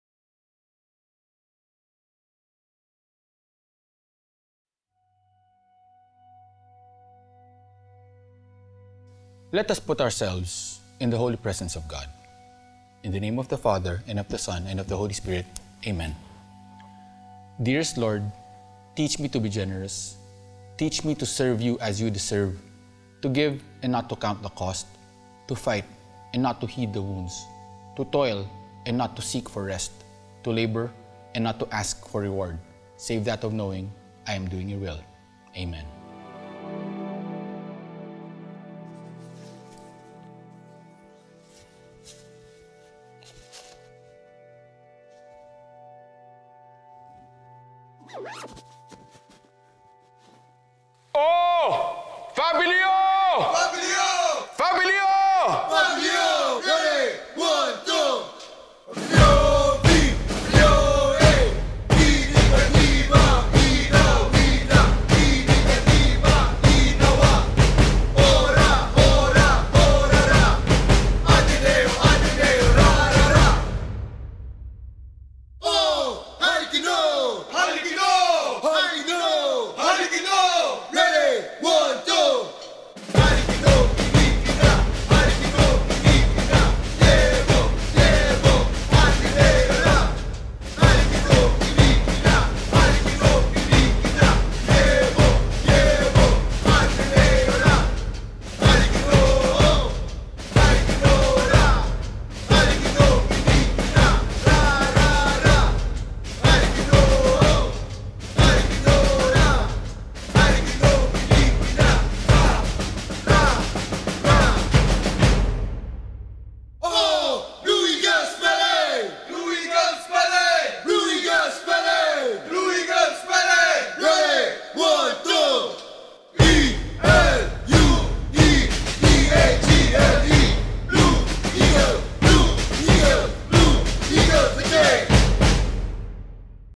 Ateneo Magis 2020 Hopecoming: Chito Miranda + Blue Babble Battalion
Chito Miranda of Ateneo Grade School 1990 and High School 1994 opens the Ateneo Magis 2020 Hopecoming with a prayer followed by leading the Ateneo Blue Babble Battalion (he was a former Co-President of the BBB).

The Magis 2020 Grand Alumni Hopecoming was created for the benefit of Tulong Sulong, a fundraising program that provides livelihood and employment opportunities for COVID-affected communities.